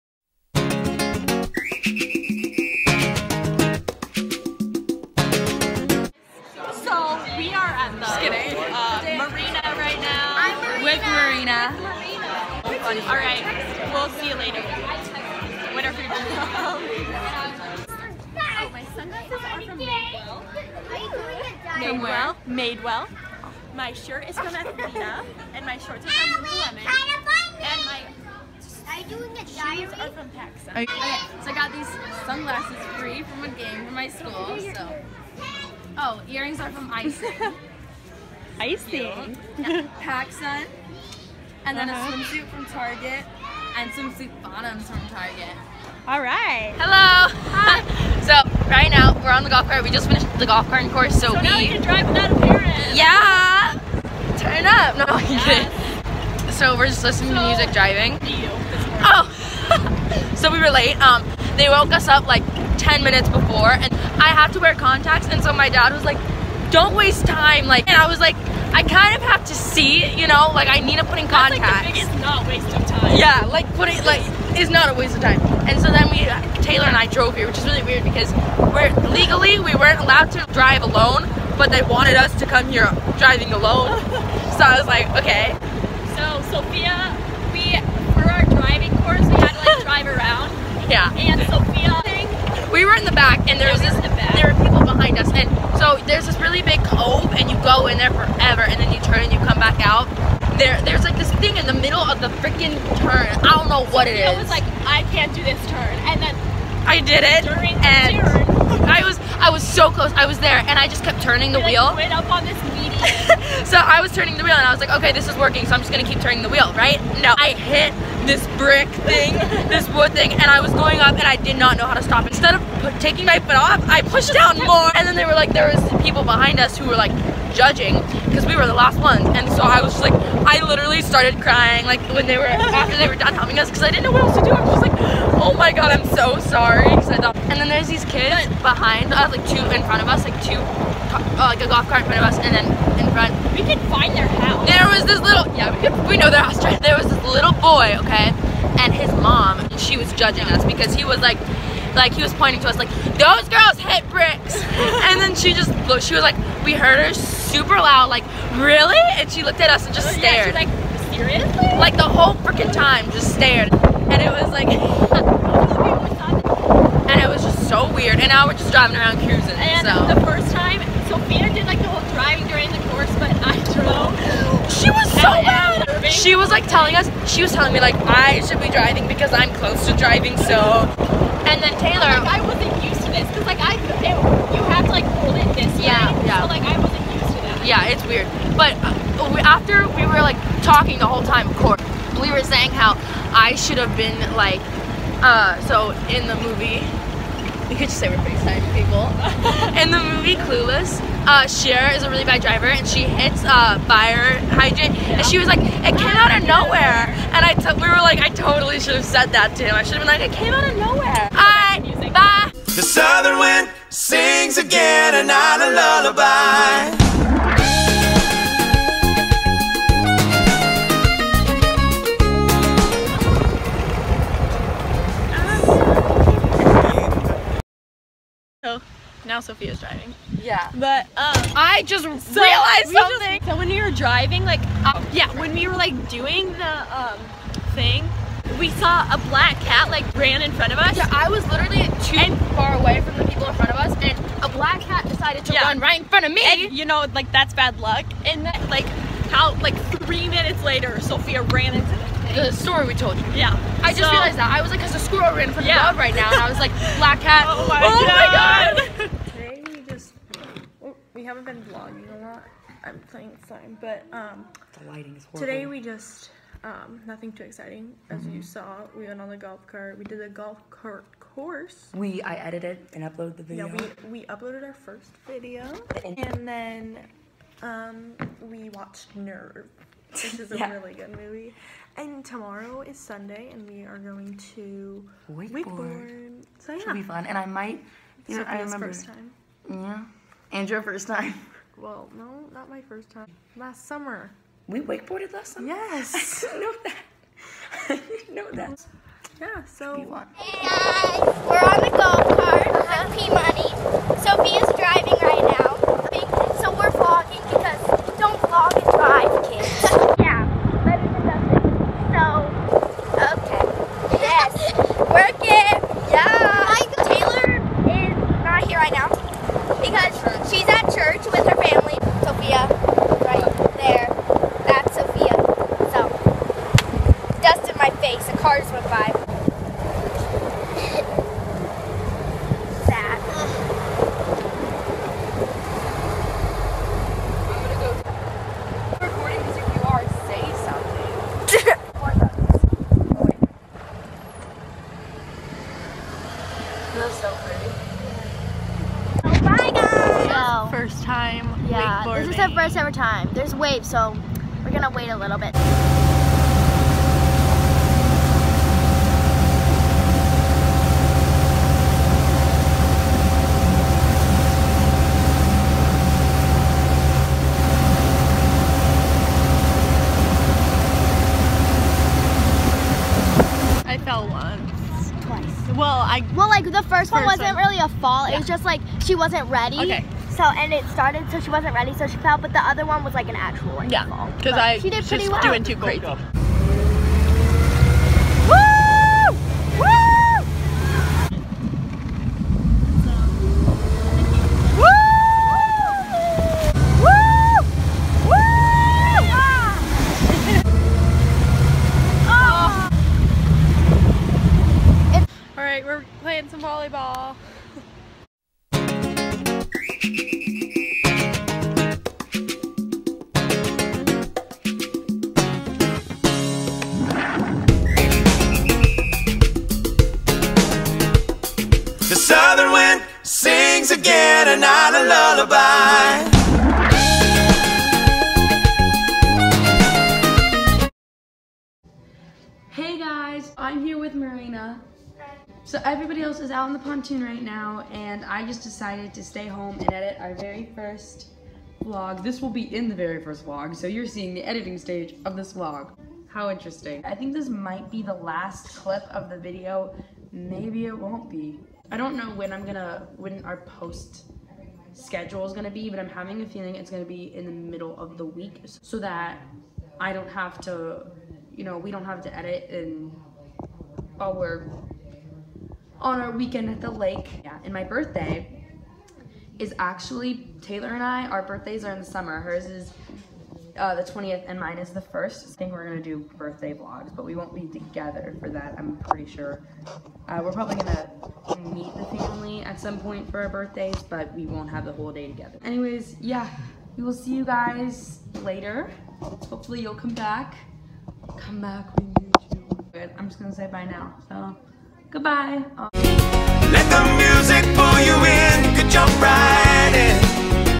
Vlog #1 | Memorial Day Weekend
This weekend we went on the golfcart, wakeboard and more.